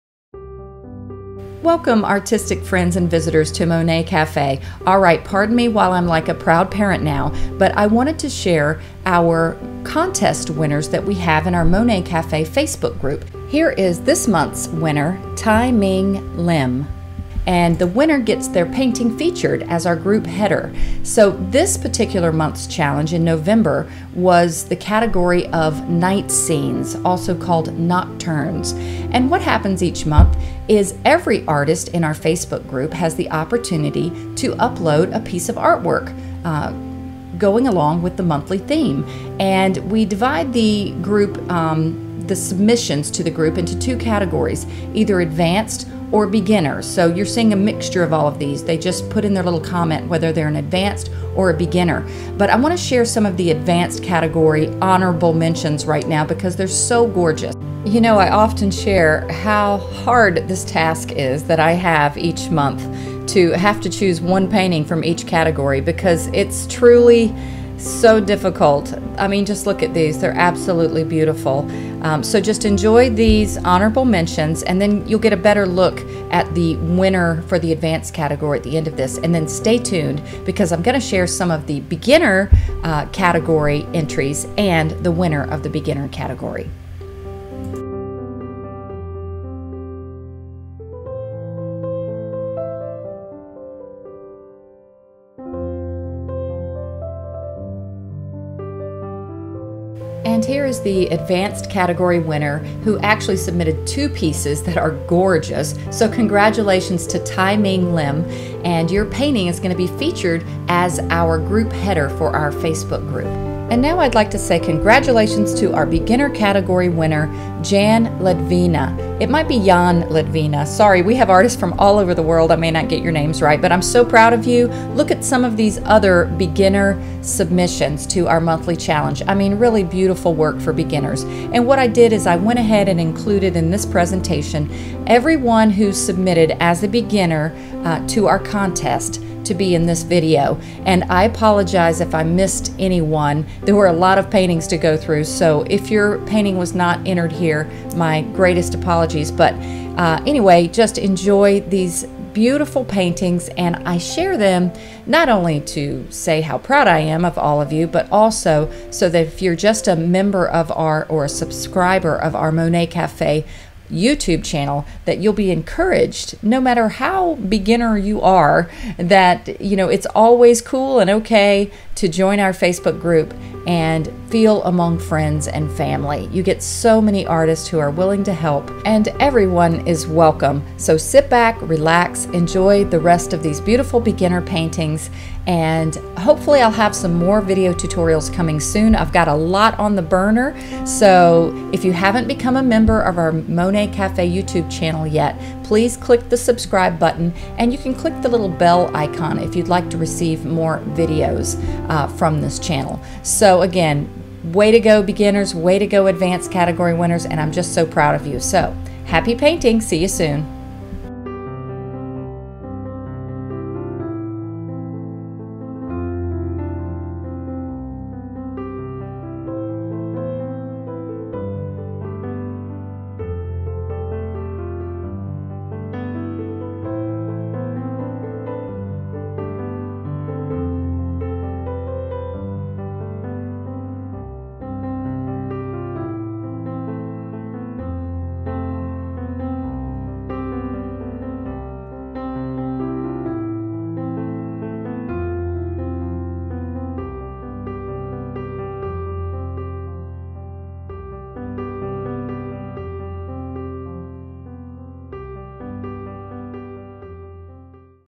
Monthly Contest Winners for our FaceBook Group!
I'm so grateful and proud of every artist in YouTube channel and Facebook Group! I wanted to share the monthly contest winners for November and hopefully encourage others to join our FB group (Monet Cafe' Art Group). It's really a beautiful family of artists of every level. Enjoy this presentation and congrats again to all of the artists who submitted pieced for the contest!!

(join for just $5 a month, cancel anytime)

▽ Monet Cafe' Artists Bracelets (from Etsy Store)👇

▽ My Book on Amazon👇"The Mountain Queen"